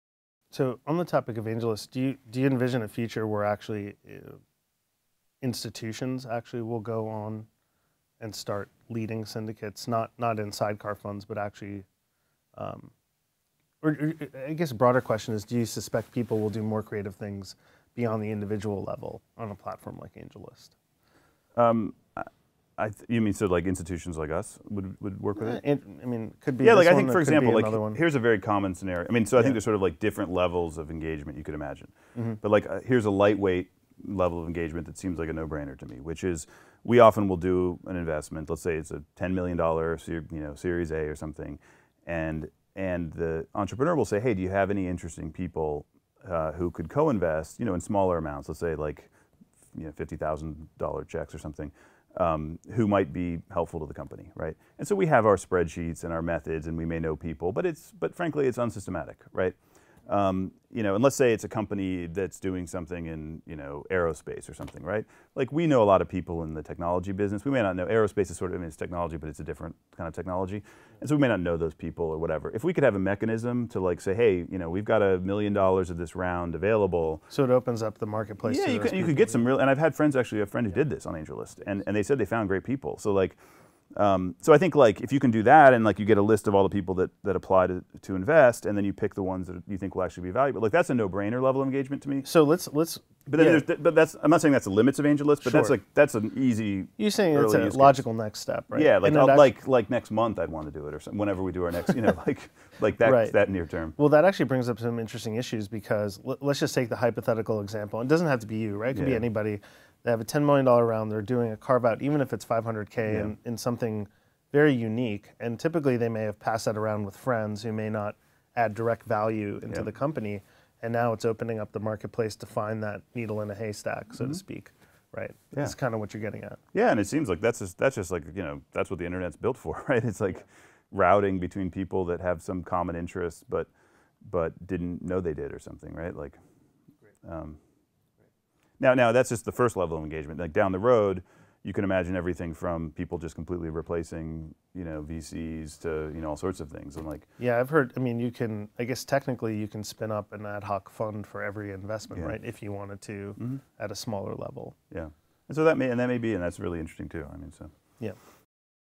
Sunday Conversation with Chris Dixon, a16z (3 of 8)
Semil Shah sits down for a conversation with Chris Dixon from a16z on Haywire.